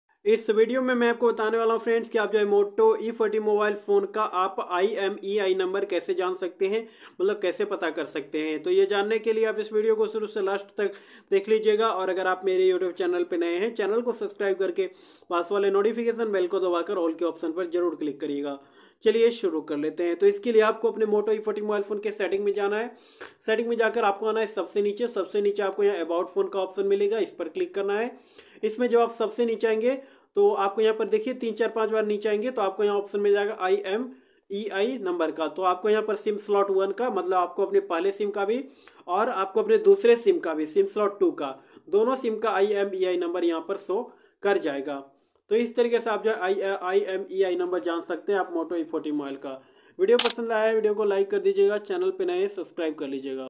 moto e40 phone ka imei number kaise pata kare ।how to find imei number of moto e40 phone
is video me bataya gaya hai friends ki moto e40 phone ka imei number kaise pata kiya jata hai